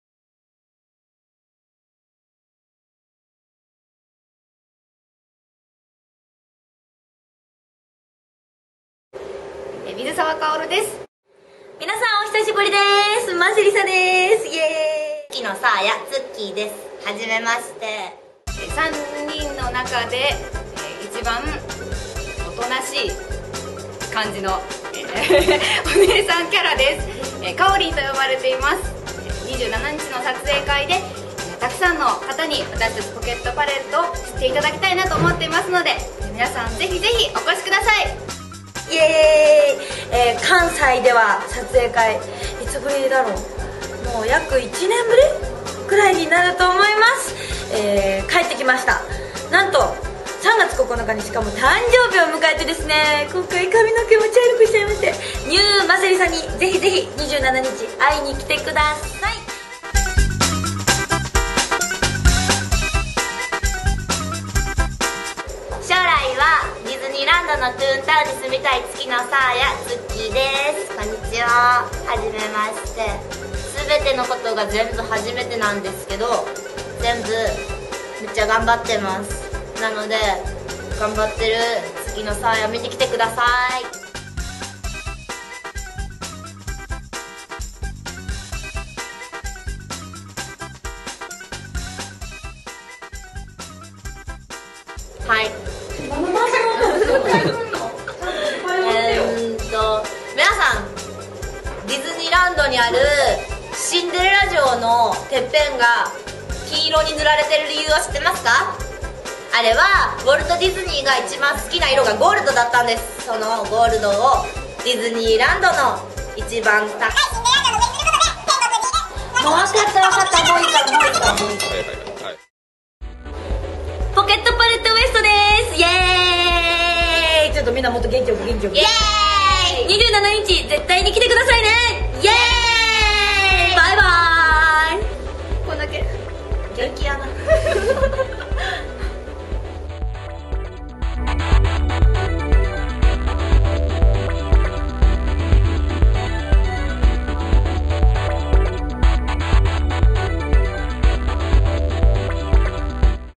ポケットパレットｗ.（水澤薫,間瀬りさ,月野さあや）
関西発、歌って、踊って、笑わせるアイドル！
5/9の初Liveを前に、初お披露目の撮影会を実施！
ご挨拶価格の各部2,500円！！